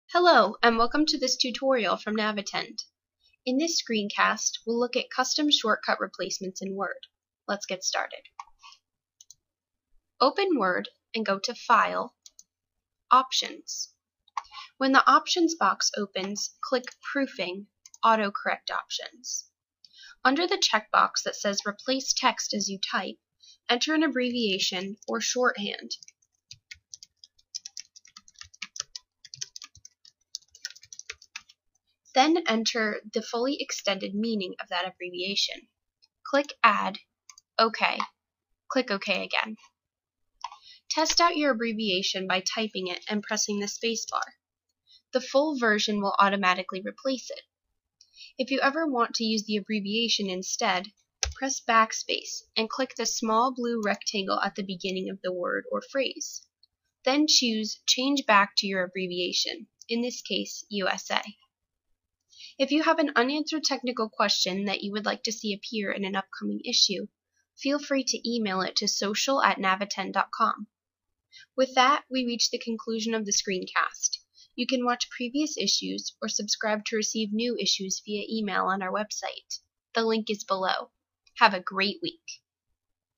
How to Create Custom Shortcut Replacements in Word
Used to shortcuts being replaced when you type a text message? You can enter shortcuts in Word's AutoCorrect settings, and your abbreviations will expand automatically.